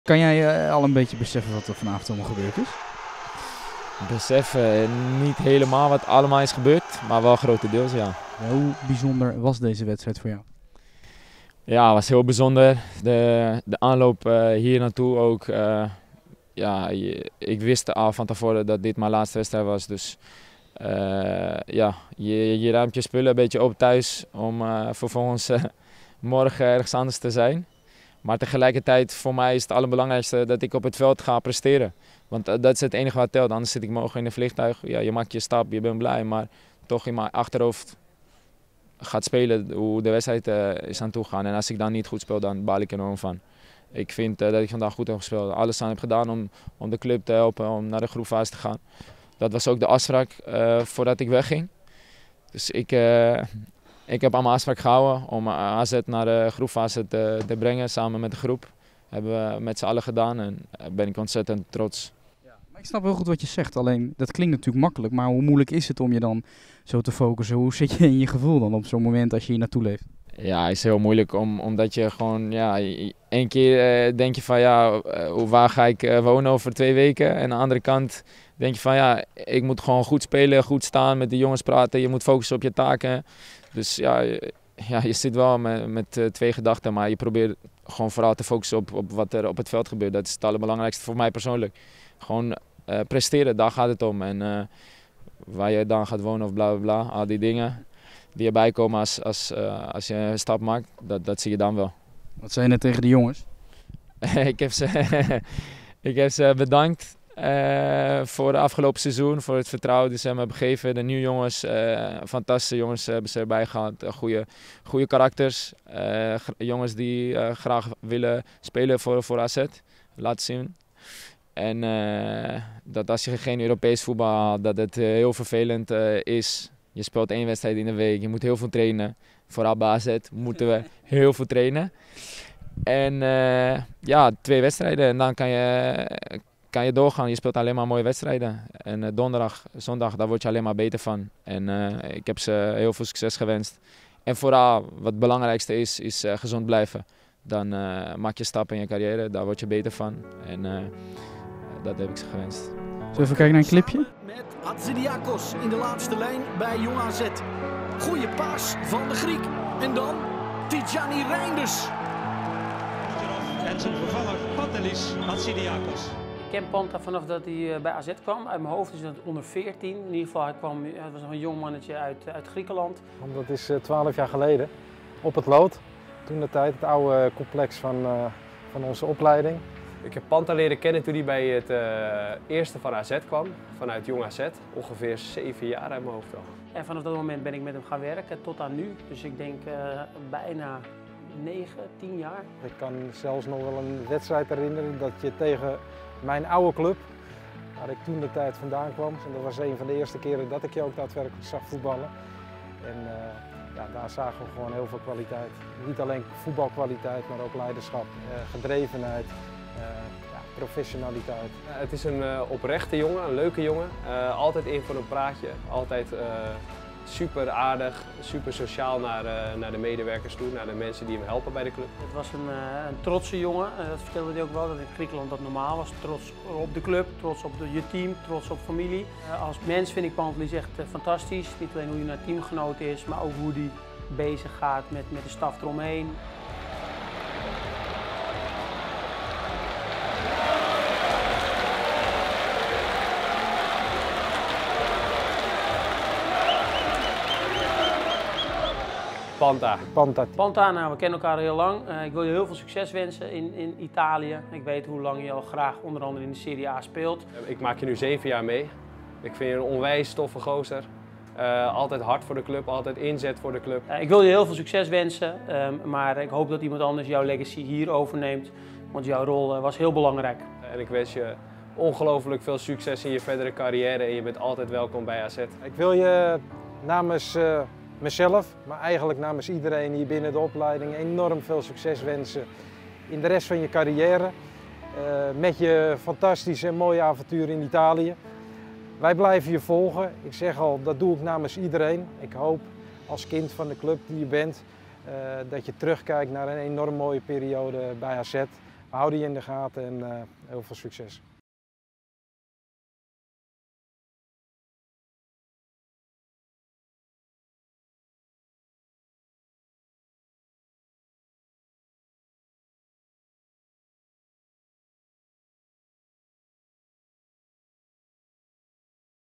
❤️ Hatzidiakos: 'Ben ontzettend trots' | Afscheidsinterview
Pantelis Hatzidiakos speelde na meer dan tien jaar tegen SK Brann zijn laatste wedstrijd voor AZ. Een afscheidsinterview met het jeugdexponent.